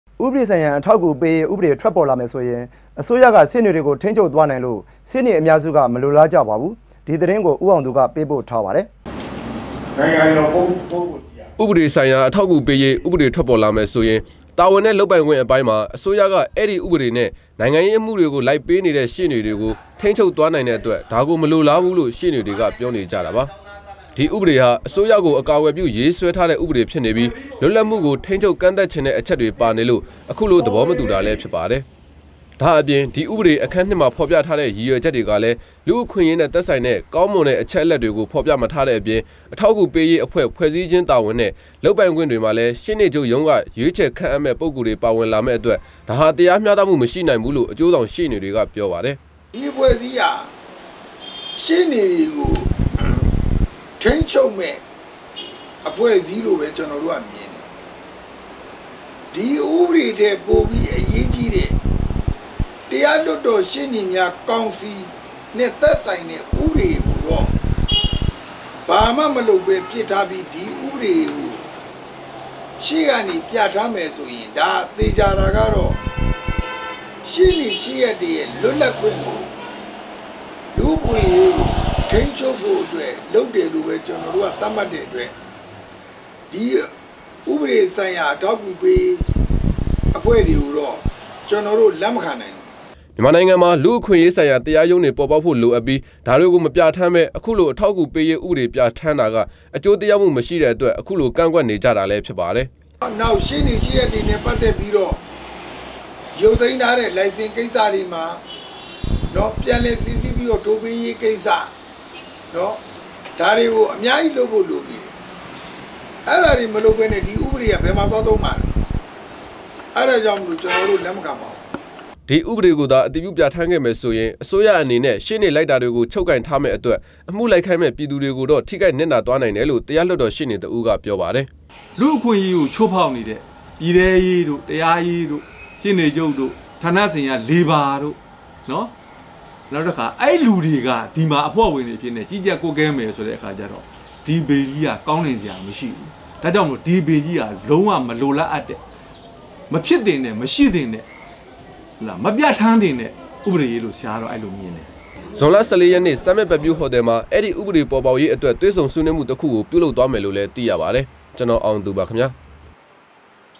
DVB - ဥပေဒဆိုင္ရာ အေထာက္အကူေပးေရး ဥပေဒက ေရွ႕ေနေတြကုိထိန္းခ်ဳပ္သြားႏုိင္တာမုိ႔ မလုိလား
DVB TV - ဥပေဒဆိုင္ရာ အေထာက္အကူေပးေရး ဥပေဒထြက္ေပၚလာမယ္ဆိုရင္ အစိုးရကေရွ႕ေနေတြကို ထိန္းခ်ဳပ္သြားႏိုင္လို႔ ေရွ႕ေနအမ်ားစုမလိုလားၾကပါဘူး။ ဒီသတင္းကို ဦးေအာင္သူကေပးပို႔ထားတာပါ။